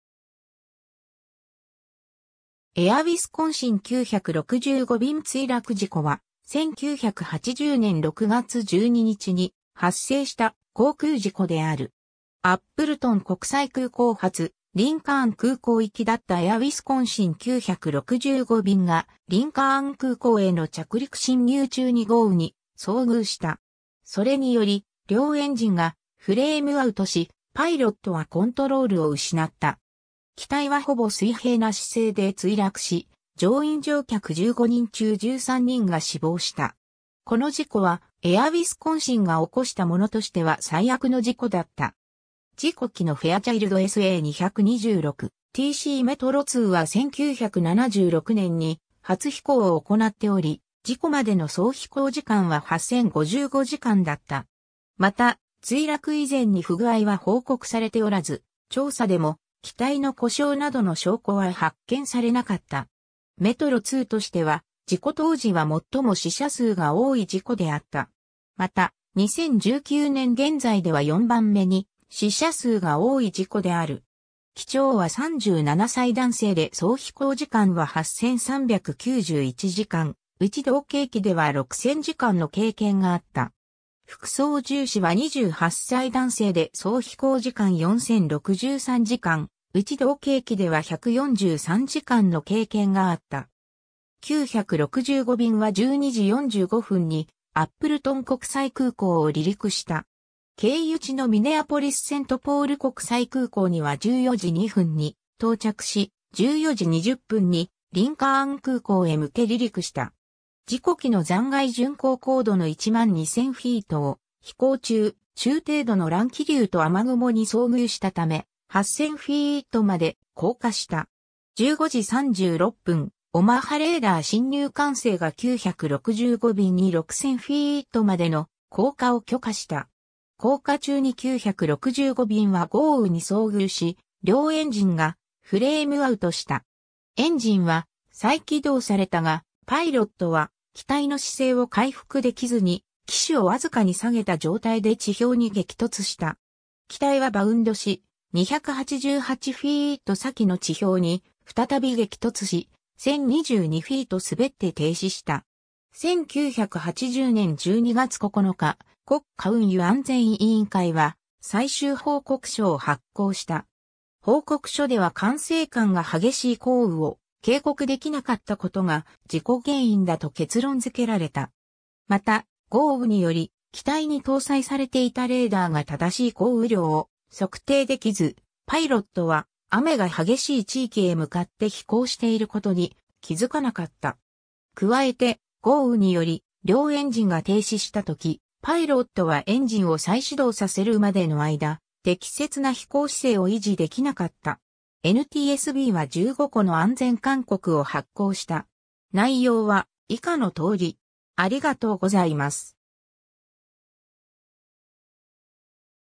エア・ウィスコンシン965便墜落事故
エア・ウィスコンシンの航空事故
エア・ウィスコンシン965便墜落事故（エア・ウィスコンシン965びんついらくじこ）は、1980年6月12日に発生した航空事故である。
アップルトン国際空港発リンカーン空港行きだったエア・ウィスコンシン965便（フェアチャイルド SA226-TC メトロⅡ ）が、リンカーン空港への着陸進入中に豪雨に遭遇した。
それにより両エンジンがフレームアウトし、パイロットはコントロールを失った。
機体はほぼ水平な姿勢で墜落し、乗員乗客15人中13人が死亡した。
この事故はエア・ウィスコンシンが起こしたものとしては最悪の事故だった。
 事故機のフェアチャイルド SA226-TC メトロⅡ （N650S）は1976年に初飛行を行っており、事故までの総飛行時間は8,055時間だった。
また、墜落以前に不具合は報告されておらず、調査でも機体の故障などの証拠は発見されなかった。
メトロⅡとしては、事故当時は最も死者数が多い事故であった。
また、2019年現在では4番目に死者数が多い事故である。
 機長は37歳男性で総飛行時間は8,391時間、うち同型機では6,000時間の経験があった。
副操縦士は28歳男性で総飛行時間4,063時間、うち同型機では143時間の経験があった。
 965便は12時45分にアップルトン国際空港を離陸した。
経由地のミネアポリス・セントポール国際空港には14時02分に到着し、14時20分にリンカーン空港へ向け離陸した。
 事故機の残骸 巡航高度の12,000フィート (3,700 m)を飛行中、中程度の乱気流と雨雲に遭遇したため、8,000フィート (2,400 m)まで降下した。
15時36分、オマハレーダー進入管制が965便に6,000フィート (1,800 m)までの降下を許可した。
降下中に965便は豪雨に遭遇し、両エンジンがフレームアウトした。
エンジンは再起動されたが、パイロットは機体の姿勢を回復できずに、機首を僅かに下げた状態で地表に激突した。
機体はバウンドし、288フィート (88 m)先の地表に再び激突し、1,022フィート (312 m)滑って停止した。
 1980年12月9日、国家運輸安全委員会（NTSB）は最終報告書を発行した。
報告書では管制官が激しい降雨を警告できなかったことが事故原因だと結論付けられた。
また、豪雨により機体に搭載されていたレーダーが正しい降雨量を測定できず、パイロットは雨が激しい地域へ向かって飛行していることに気づかなかった。
加えて、豪雨により両エンジンが停止したとき、パイロットはエンジンを再始動させるまでの間、適切な飛行姿勢を維持できなかった。
 NTSBは15個の安全勧告を発行した。
内容は以下の通り。